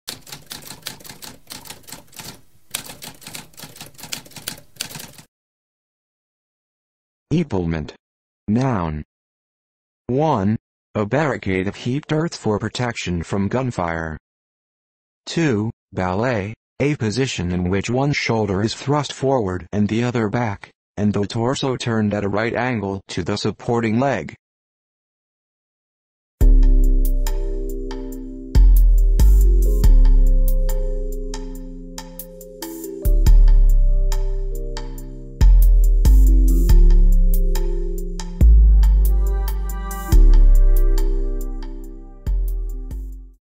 What does epaulement mean?
A spoken definition of epaulement.

Intro Sound:
Typewriter - Tamskp